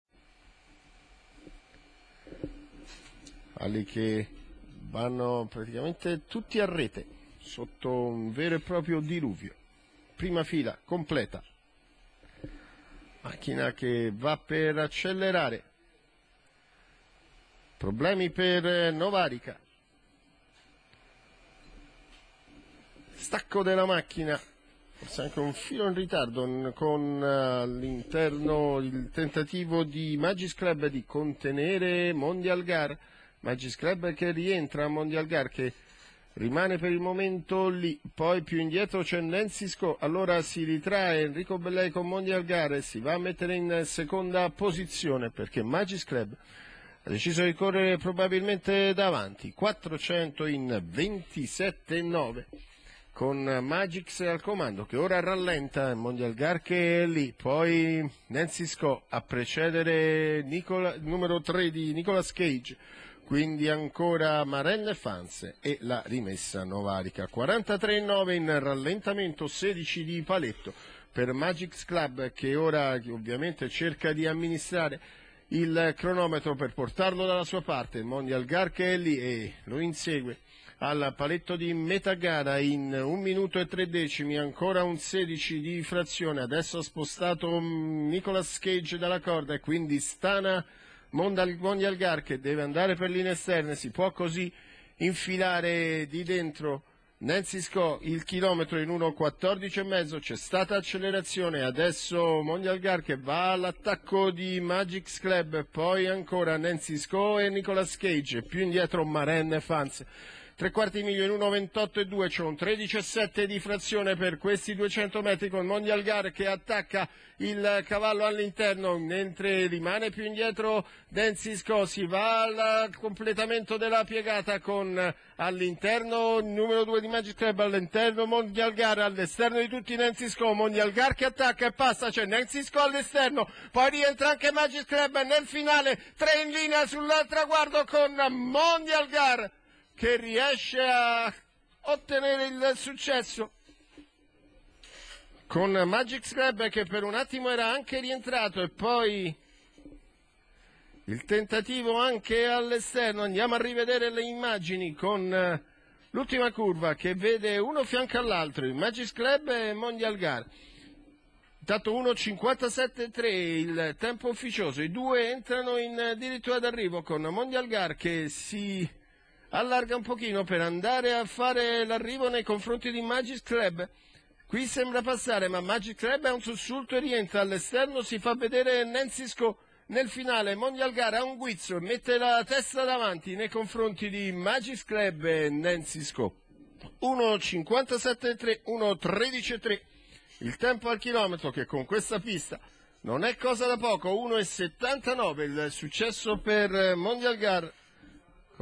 31/10/2012, Roma , Premio Pakistan
1. MONDIAL GAR (BELLEI ENRICO), 2. MAGIC'S CLUB (DI LORENZO TOMMASO), 3. NANCY SCO (CARTA PAOLO), 4. MARENNE FANS (MINNUCCI GIAMPAOLO), 5. NICHOLAS CAGE (BECCHETTI LUCIO), 99. NOVARICA (GOCCIADORO ALESSANDRO)